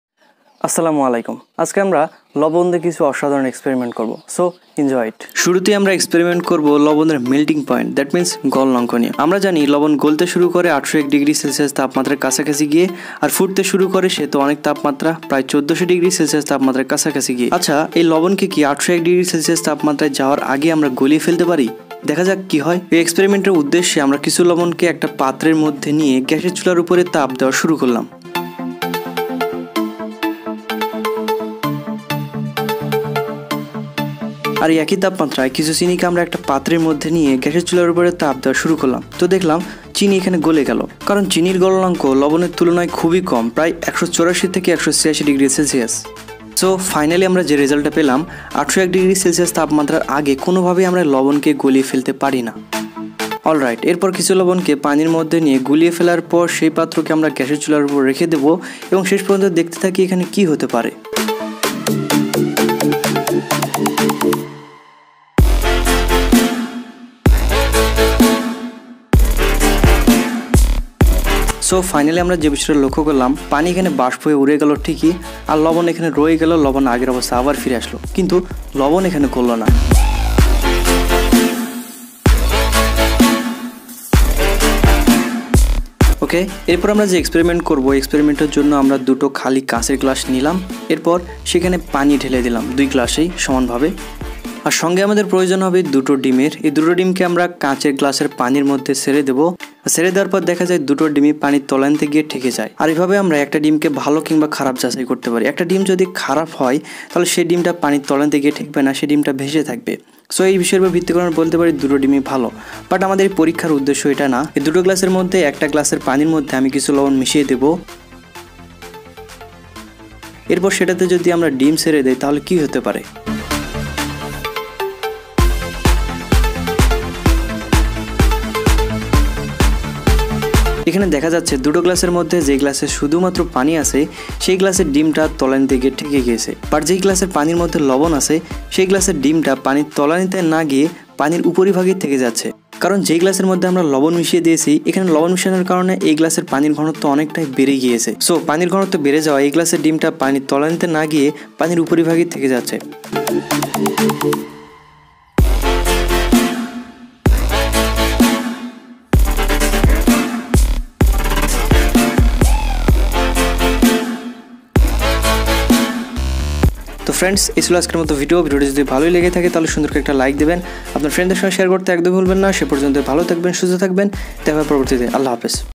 লবন দিয়ে অসাধারণ কিছু এক্সপেরিমেন্ট | Amazing Salt Experiment At Home
Experiment 1:
Can we melt salt under 801°C Temperature?

Experiment 2:
What happen if we mix salt and water during that heating time?

Experiment 3:
Egg and Salt Experiment

Experiment 4:
Kerosene and Salt Experiment